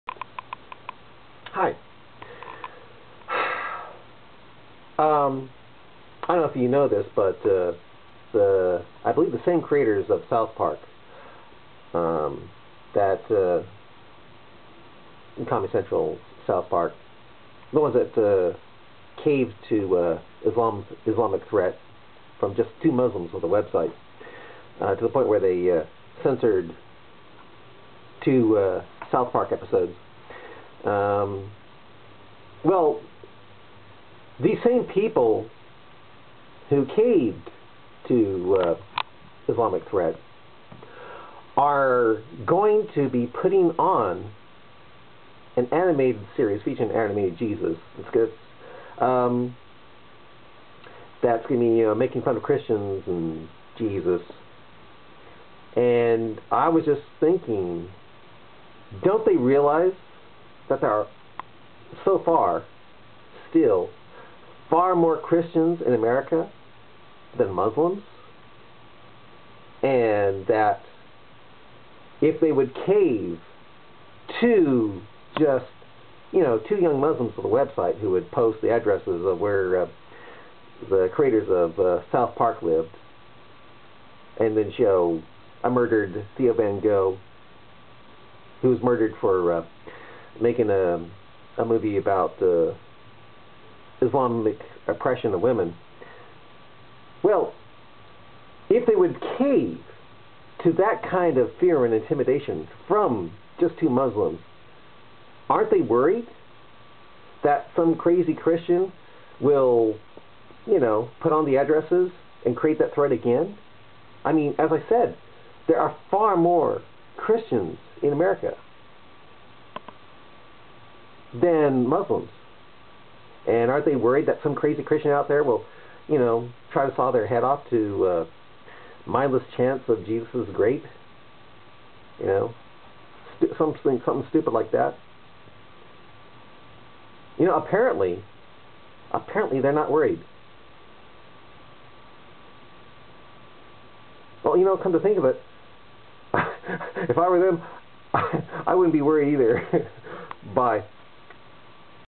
Hey, Isn't Comedy Central Worried about Christian Back-lash With "The Jesus Show"?
Hey, isn't Comedy Central worried about a strong Christian back-lash from it's "Jesus Christ" show when there are still FAR more Christians in America than Muslims?  After all, it only took just TWO young Muslims with a web site to get Comedy Central to censor South Park when these Muslims posted the addresses of South Park creators and showed a murdered The Van Gogh for making a film showing the Islamic oppression of women.